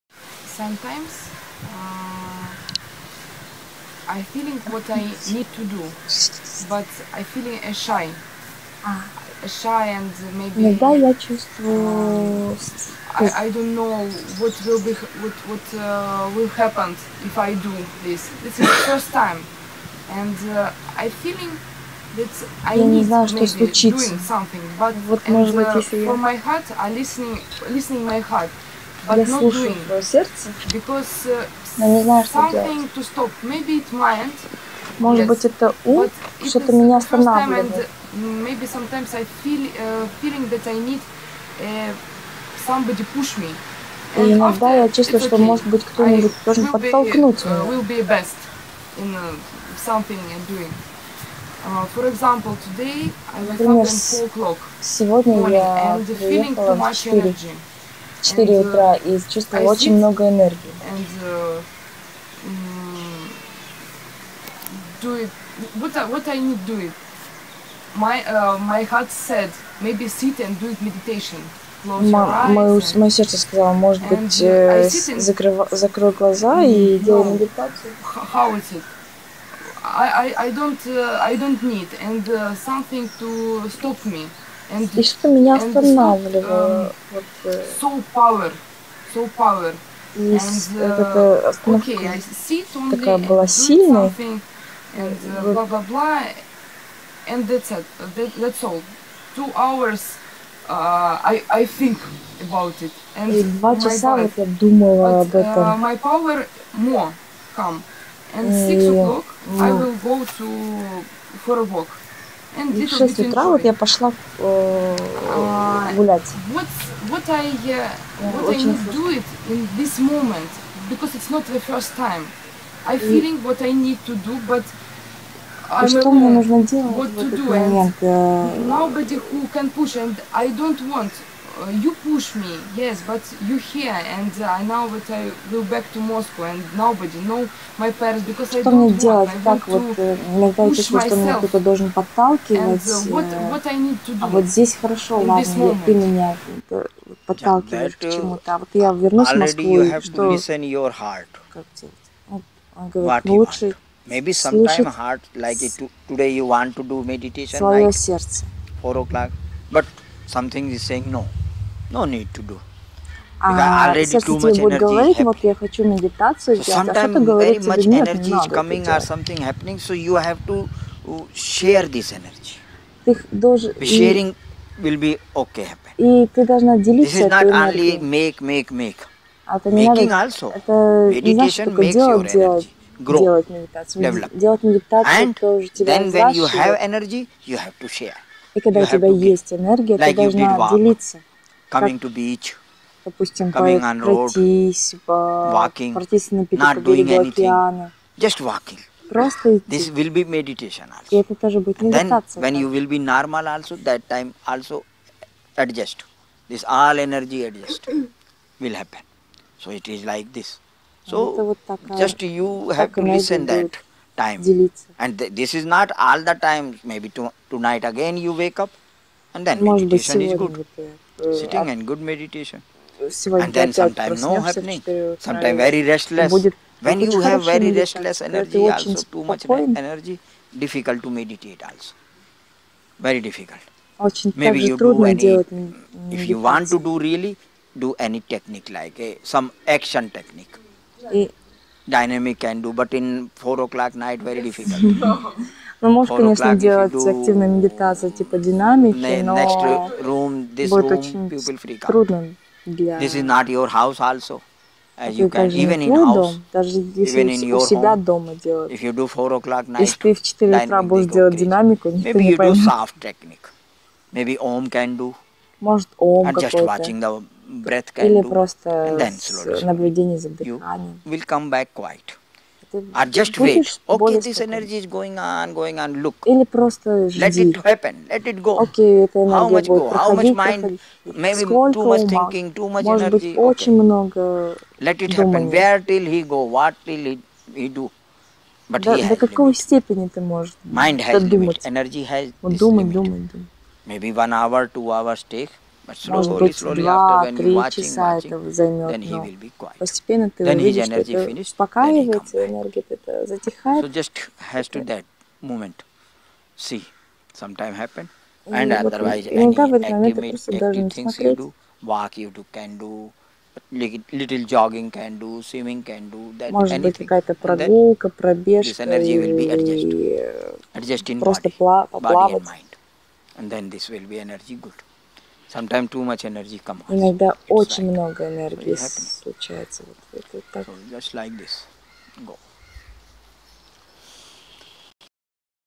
12.02.2018 часть 2. Что меня не пускает медитировать?
Сатсанг с Самдарши в Агонде, Гоа 12 фев 2018 часть 2
Satsang with Samdarshi in Agonda, Goa 12 feb 2018 part 2